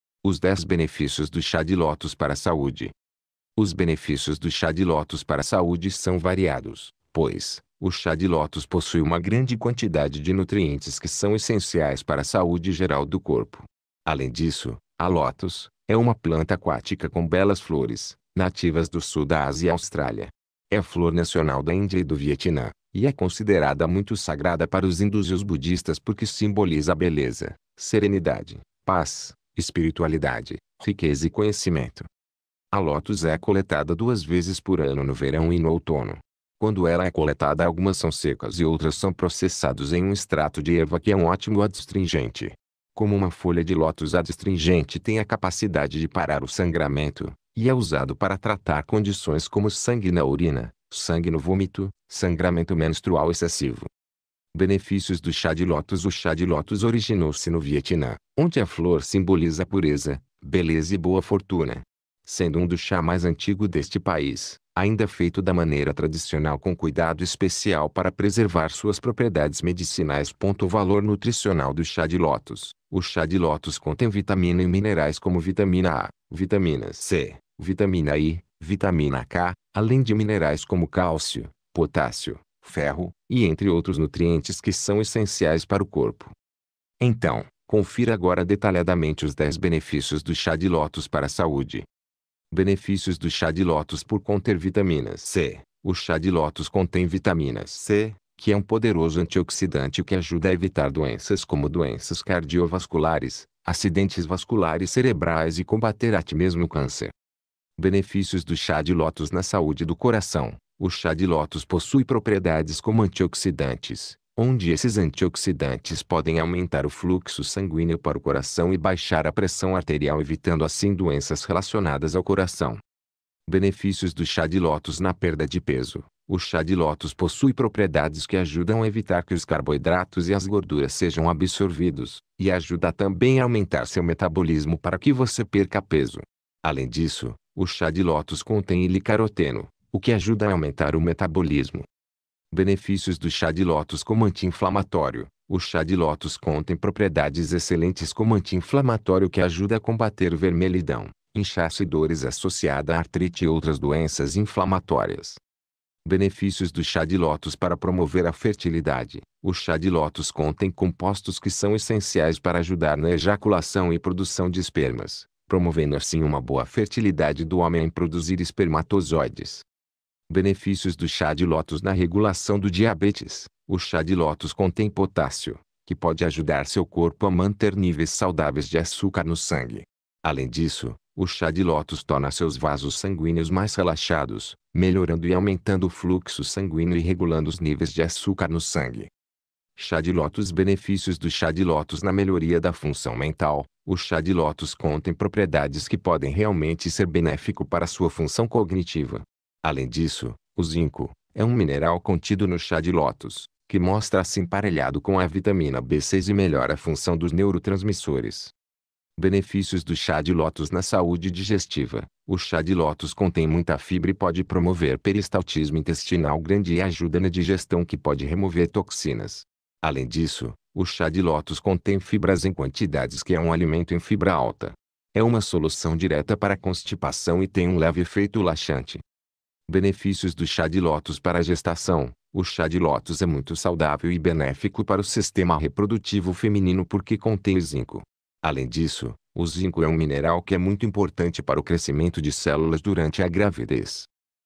Os 10 Benefícios do Chá de Lótus Para Saúde
Os Benefícios do Chá de Lótus Para Saúde são variados, pois, o Chá de Lótus possui uma grande quantidade de nutrientes que são essenciais para saúde geral do corpo. Além disso, a lótus, é uma planta aquática com belas flores, nativas do sul da Ásia e Austrália. É a flor nacional da Índia e do Vietnã, e é considerada muito sagrada para os hindus e os budistas porque simboliza beleza, serenidade, paz, espiritualidade, riqueza e conhecimento.